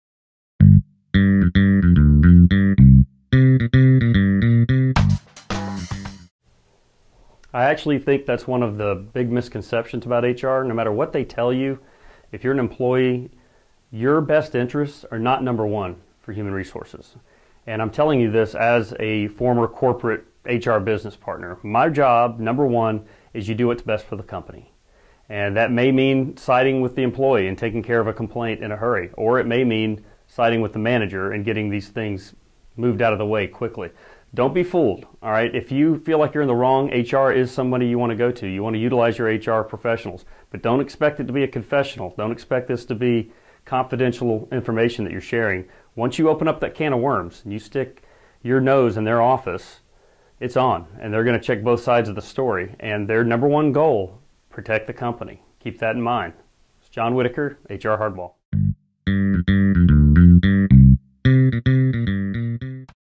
HR Hardball Video #8 Is HR there for me?
John Whitaker of HR Hardball discusses what to expect when you go to HR with an issue.